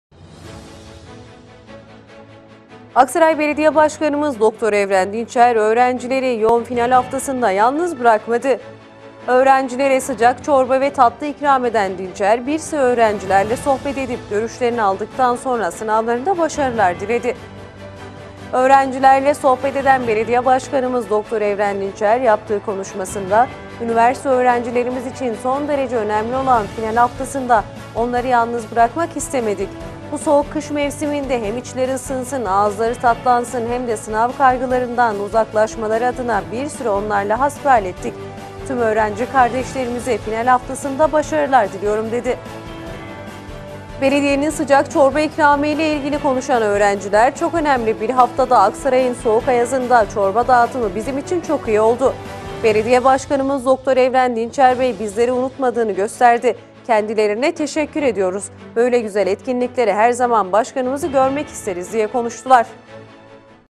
AKSARAY BELEDİYESİ'NDEN FİNAL HAFTASINDA ÖĞRENCİLERE ÇORBA VE TATLI İKRAMI
BAŞKAN DİNÇER, "TÜM ÖĞRENCİLERİMİZE BAŞARILAR DİLİYORUM"

Aksaray Belediye Başkanı Dr. Evren Dinçer öğrencileri yoğun final haftasında yalnız bırakmadı. Öğrencilere sıcak çorba ve tatlı ikram eden Dinçer, bir süre öğrencilerle sohbet edip görüşlerini aldıktan sonra sınavlarında başarılar diledi.

Soğuk kış günlerinde Aksaray Eğitim ve Artırma Hastanesi ile Sağlık İl Müdürlüğü bahçesindeki çorba evinden ve mobil çorba aracı her gün farklı noktalarda çorba hizmeti devam ederken Mobil çorba aracının bugünkü adresi ise Aksaray Üniversitesi merkez yerleşkesi oldu.

Belediye Başkanı Dr. Evren Dinçer, öğrencilerin en yoğun olduğu final haftasında onları yalnız bırakmadı. Başkan Dinçer’e Rektör Yardımcısı Prof. Dr. Ayhan Özçiftçi ve AK Parti Gençlik Kolları Başkanı Av. Şükrü Öztürk eşlik etti. Başkan ve beraberindekiler, Mobil Çorba aracından kendi elleriyle öğrencilere çorba ve tatlı ikram etti.

Öğrencilerle sohbet eden Başkan Dinçer, yaptığı konuşmasında “Üniversite öğrencilerimiz için son derece önemli olan final haftasında onları yalnız bırakmak istemedik. Bu soğuk kış mevsiminde hem içleri ısınsın, ağızları tatlansın hem de sınav kaygılarından uzaklaşmaları adına bir süre onlarla hasbihal ettik. Tüm öğrenci kardeşlerimize final haftasında başarılar diliyorum.” dedi.

Belediyenin sıcak çorba ikramı ile ilgili konuşan öğrenciler, "Çok önemli bir haftada, Aksaray’ın soğuk ayazında çorba dağıtımı bizim için çok iyi oldu. Belediye Başkanımız Dr. Evren Dinçer bey bizleri unutmadığını gösterdi. Kendilerine teşekkür ederiz. Böyle güzel etkinliklerde her zaman Başkanımızı görmek isteriz." diye konuştular.

Başkan Dinçer, öğrencilerle final haftasıyla ilgili anıları paylaştı. Sohbet ortamında devam eden etkinlik günün anısına çekilen hatıra fotoğraflarıyla son buldu.